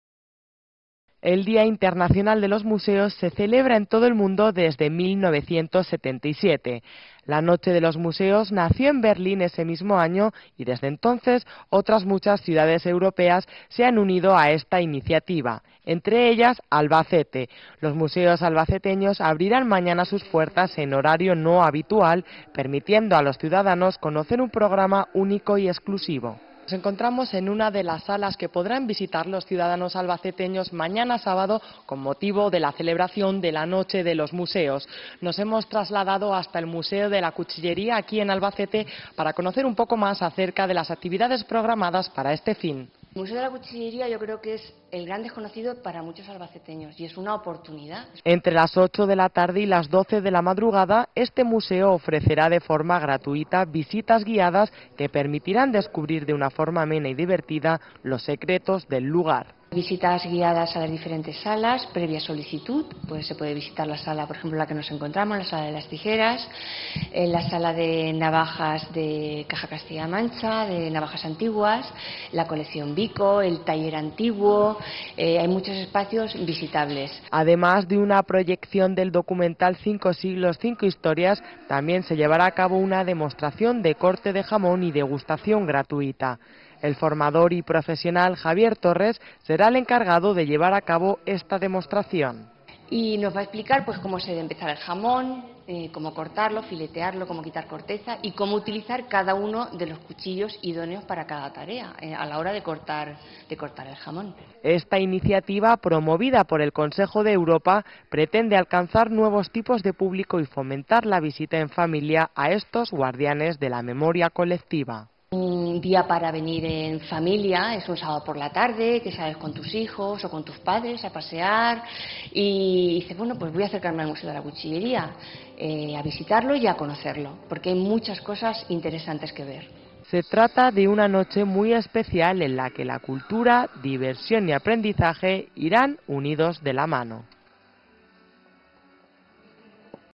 Día internacional de los museos
Una celebración que Albacete celebra con jornadas de puertas abiertas.